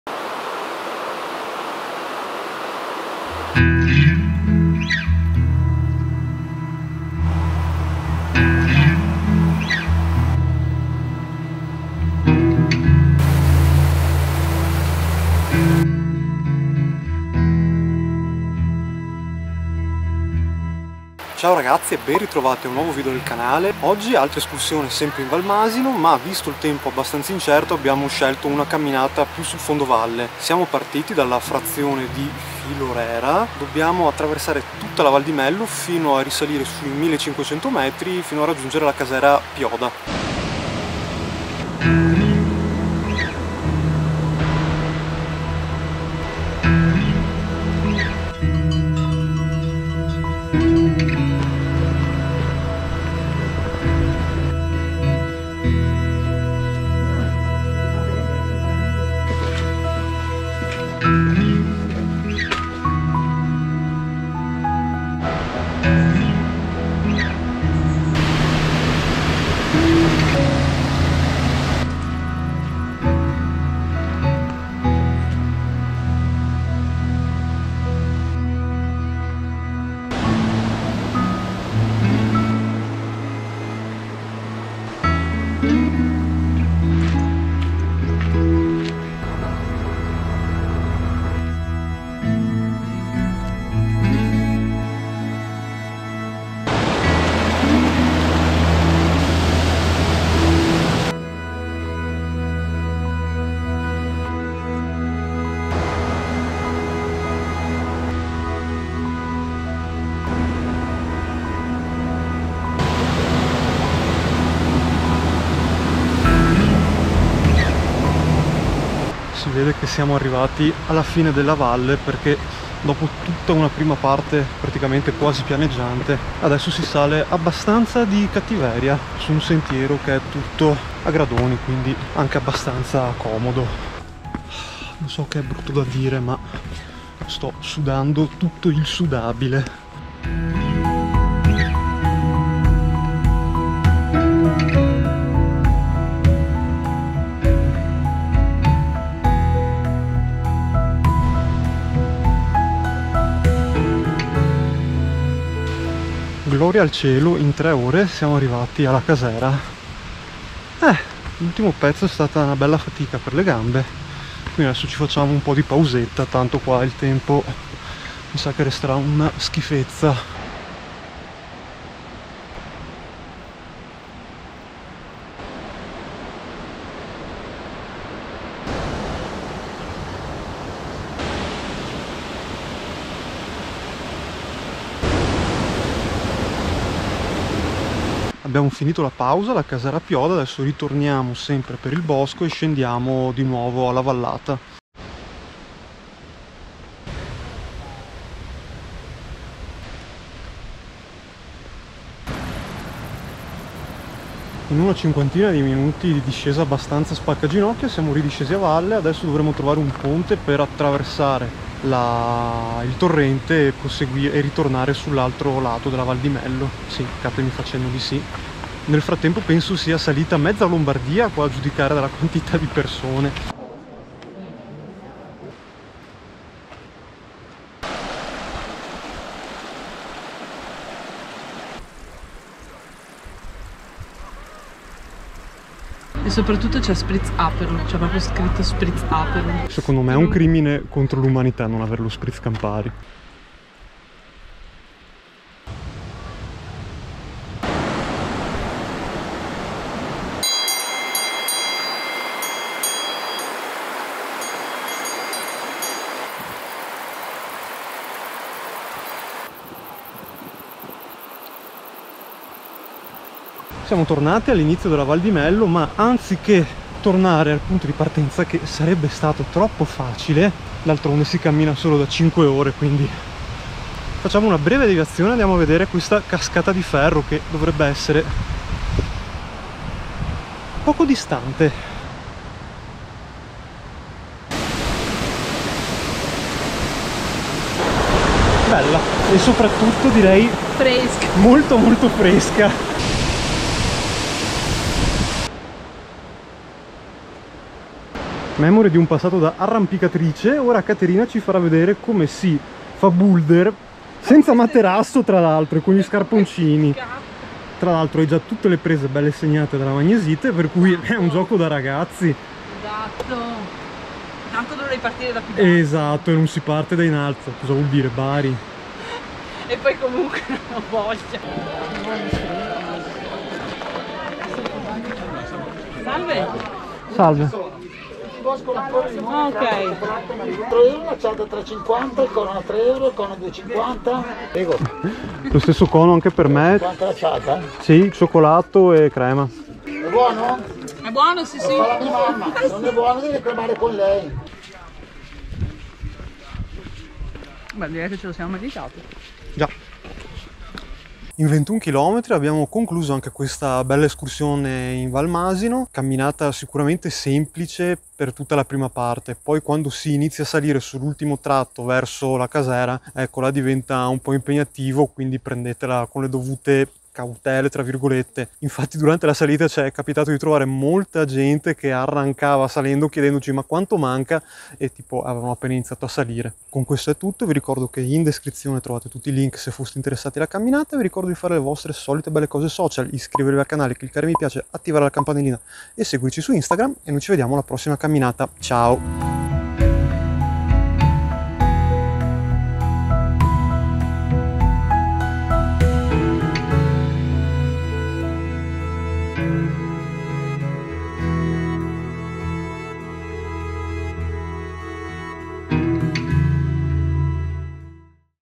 Val di Mello e Casera Pioda
Escursione nell'incantevole (e superaffolata) val di Mello e Casera Pioda

Per qualsiasi richiesta/proposta, scrivimi a

...e già che ci sei perché non ti iscrivi alla newsletter?

Video della traccia realizzata con l'app Relive